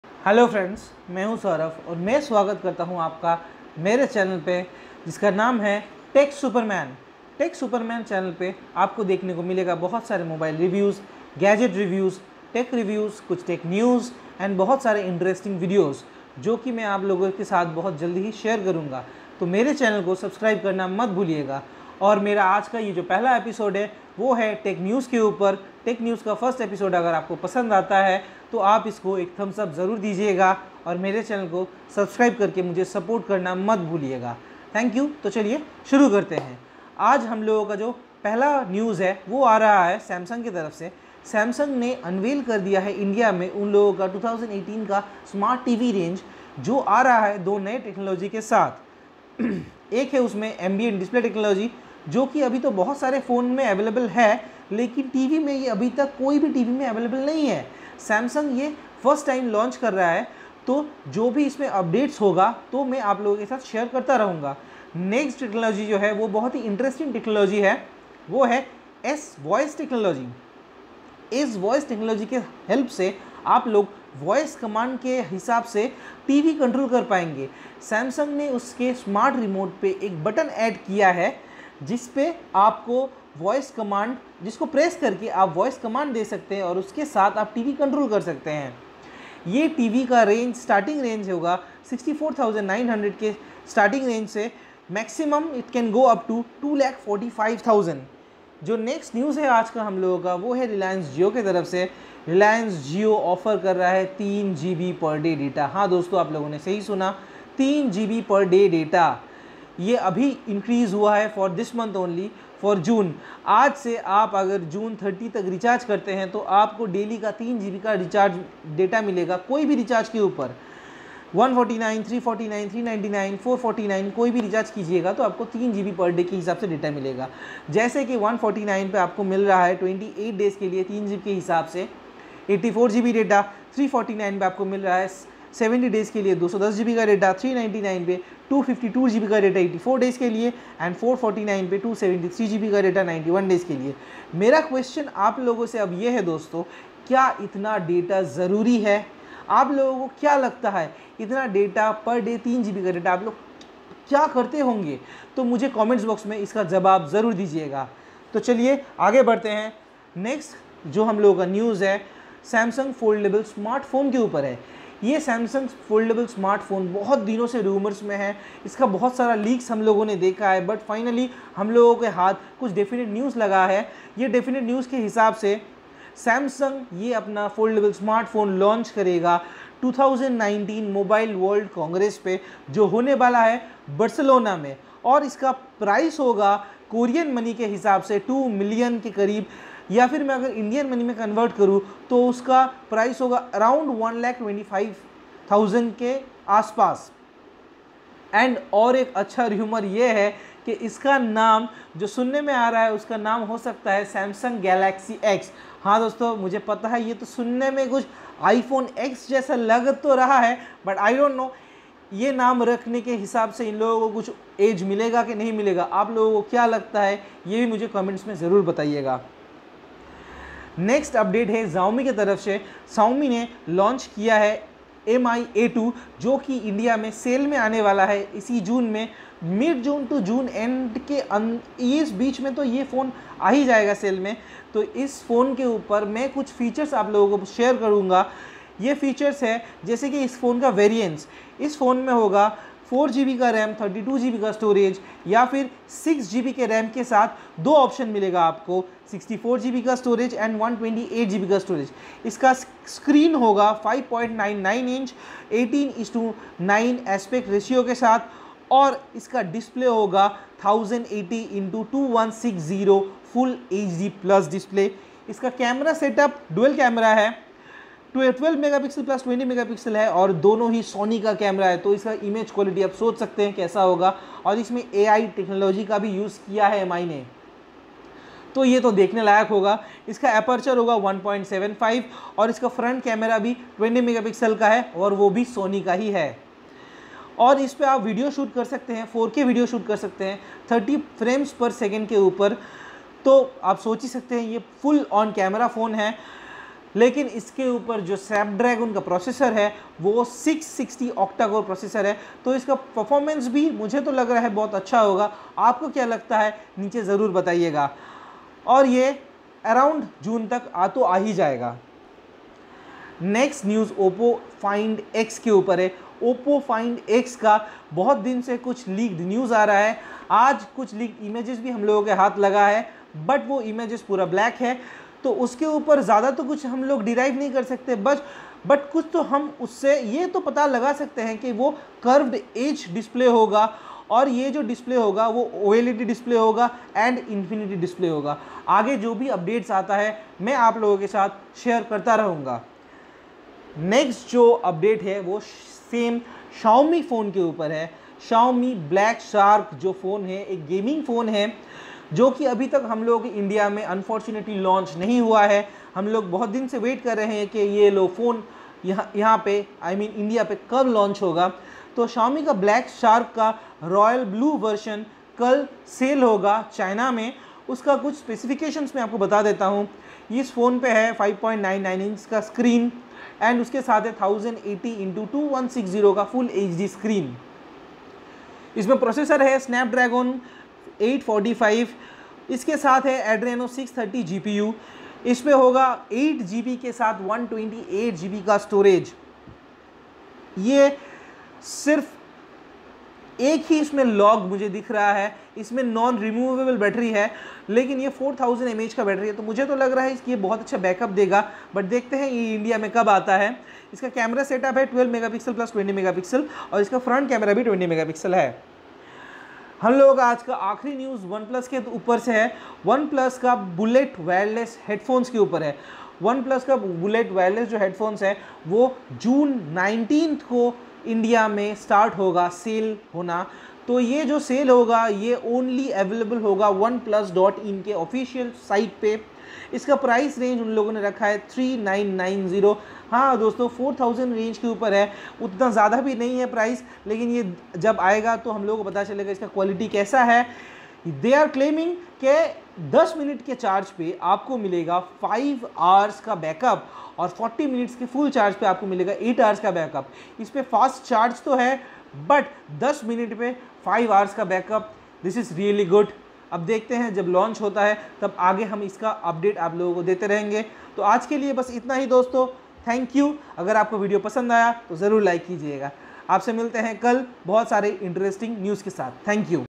15th June Tech News #1 - Samsung TV, Jio-3GB per day, Xiaomi A2 (MI 6X), One plus wireless headset
Hi Friends..